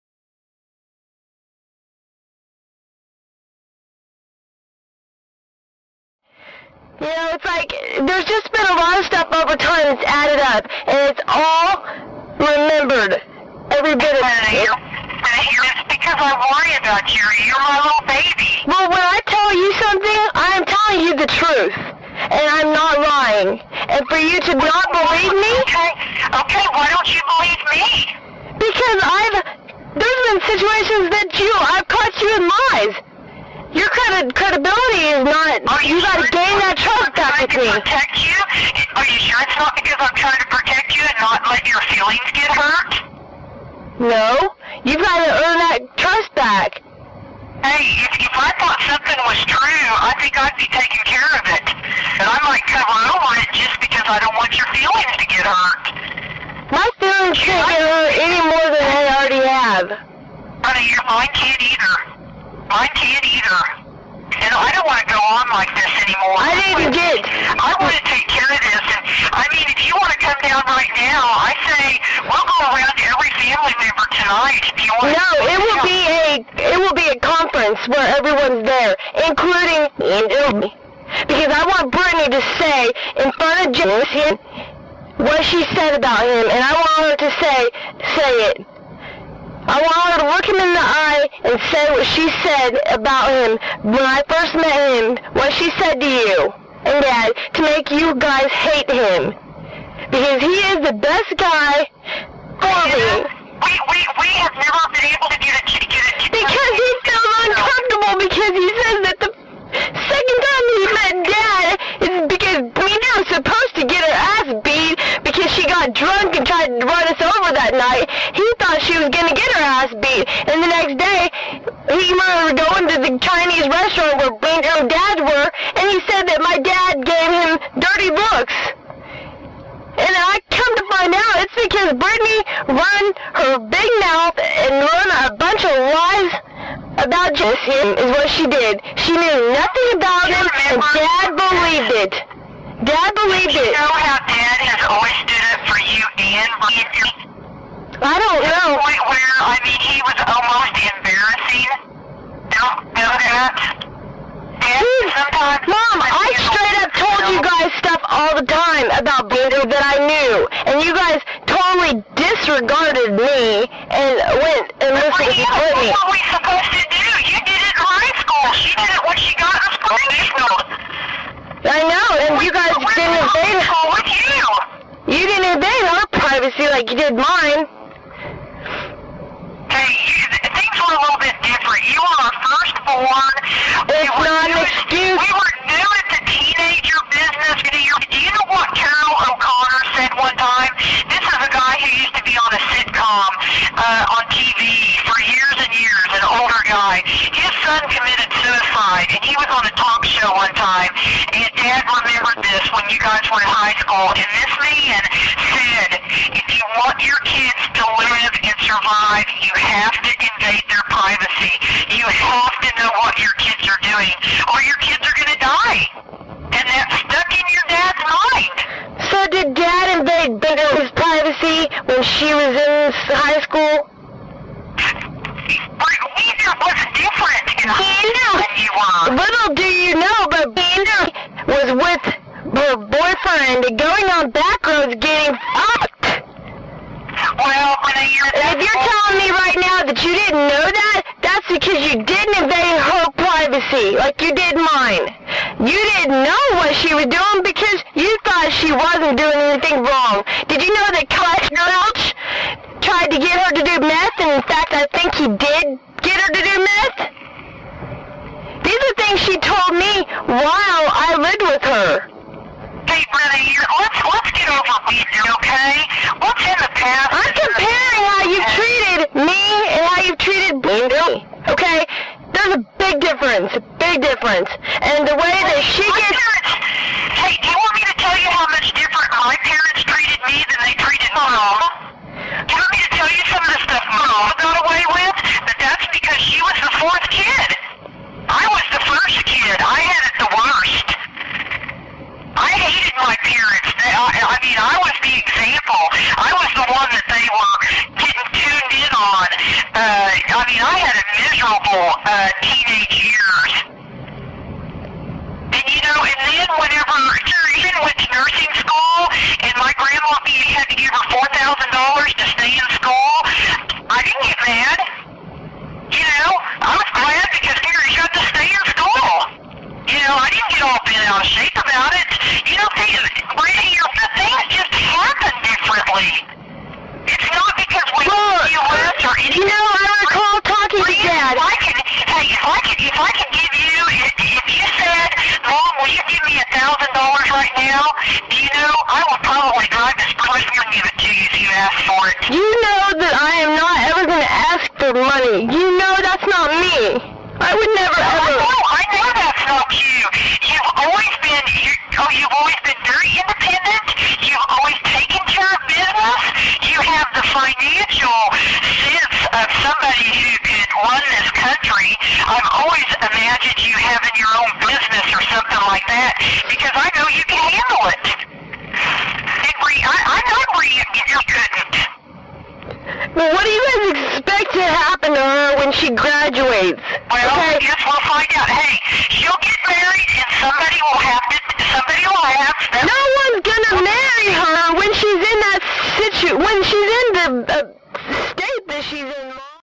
Narcissistic Mother Rewriting History | Part 3 / 3
Part 3 of this phone call is full of examples of maternal narcissism. I was still unaware of my Mom's mental problem. This was seven tears ago and she still refuses to admit the truth.